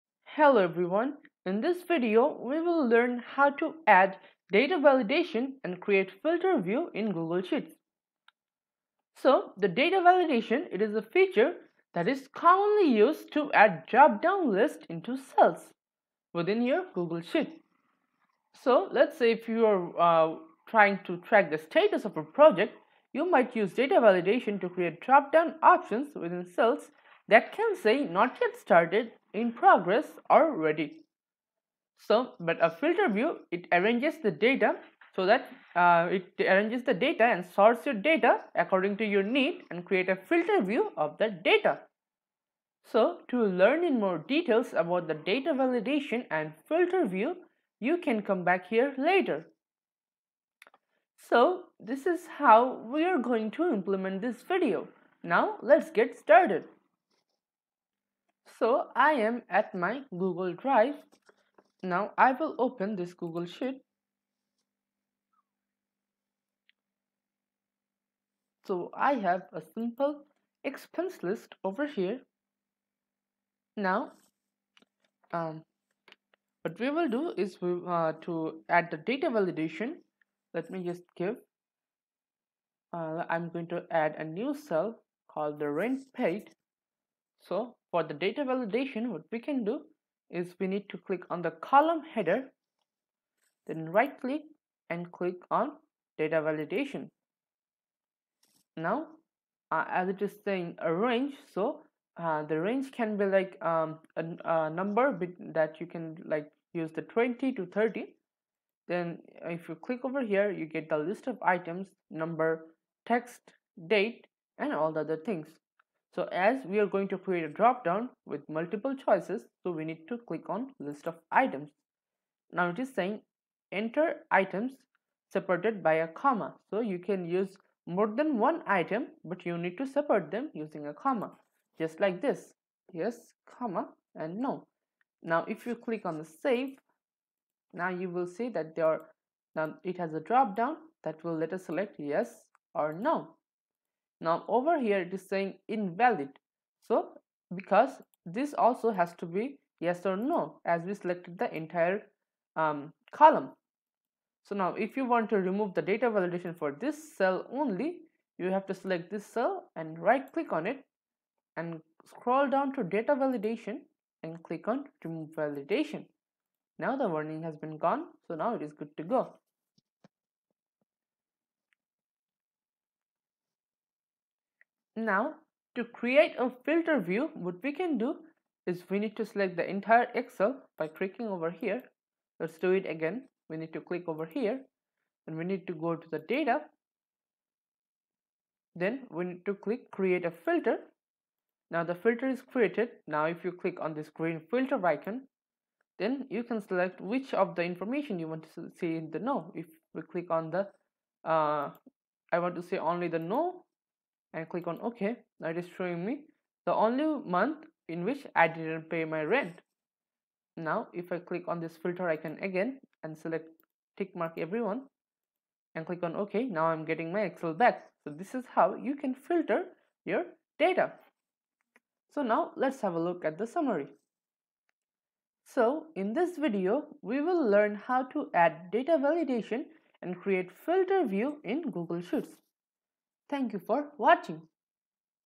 24 How to add data validation and create filter view in Google Sheets
Data Validation is a feature that's most commonly used to add dropdown lists into cells within Sheets
Let's say you want a column within your spreadsheet to track the status of a project
You might use data validation to create dropdown options within a cell that say “Not yet started,” “In progress” or “Ready”
A Filter View is a view (or arrangement of data) that you can save for future viewing/use
A filtered view sits above the original data. You can filter or sort the data according to your need and then create a 'filter view' of that data.